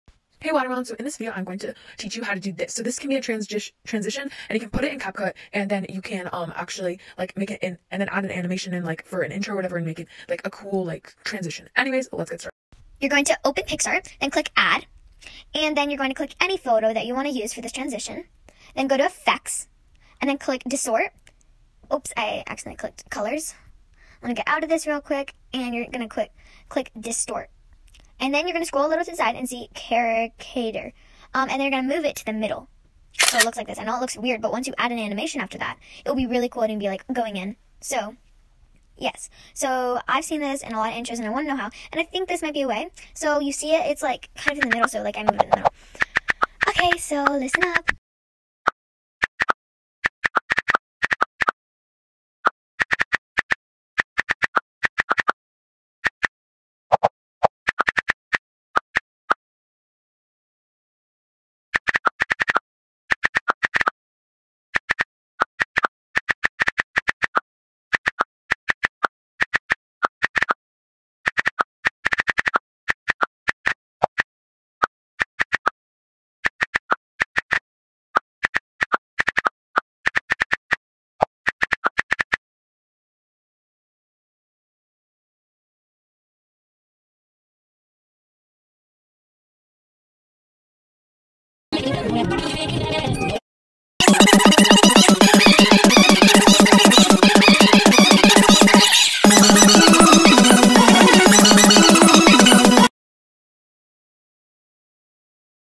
How to make this very cool transition in Picsart! (Give credits to me if you do this)
This is just something I found out and of course it’s dumb but yeah I just thought it might be cool ish? But you know those transitions people make in intros well I thought this could be one of the ways people do it!

Song: there is no song today

Editing app: Cap cut for effects)) and Picsart for the video!

Director: me

Editing person: me

Person to give credits to: me

Please give credits to me I made this idea so give credits to meee!